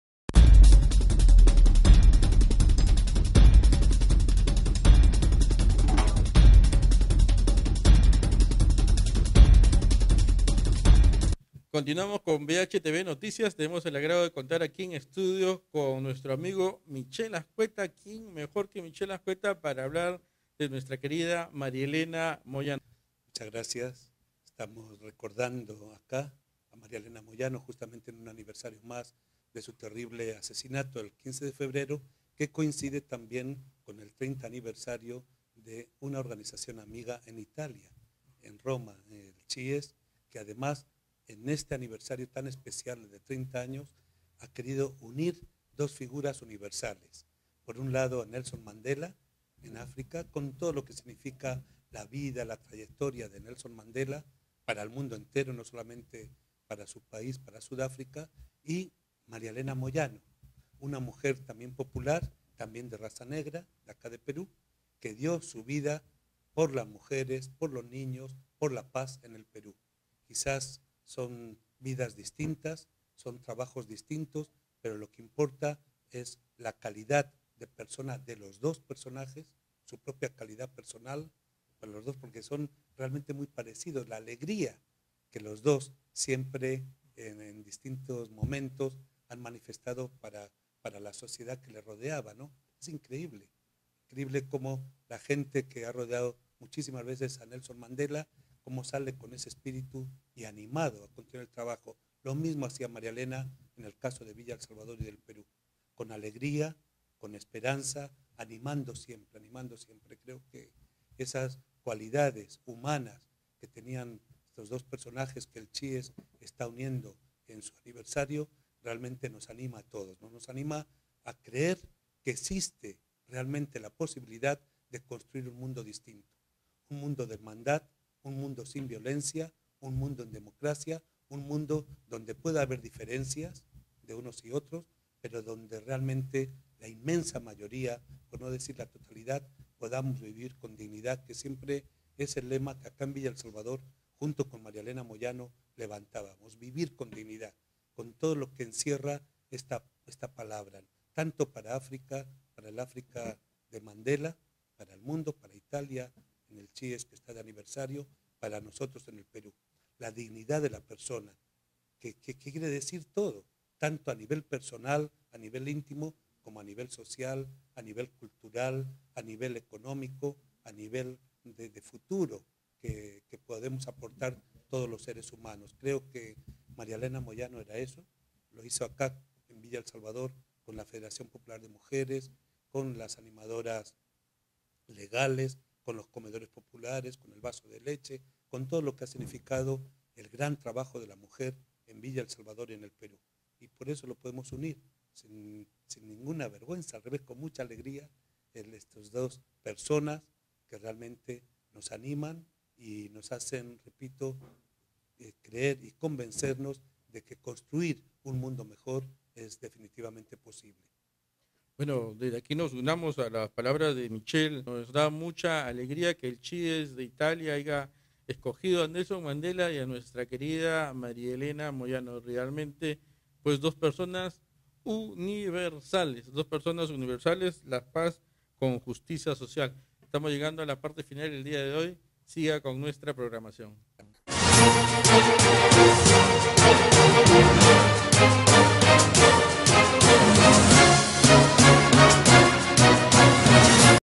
Michel Azcueta: Homenaje a Maria Elena Moyano
En los 30 años de CIES - Italia, rinden homenaje a Maria Elena Moyano, heroina nacional del Perú.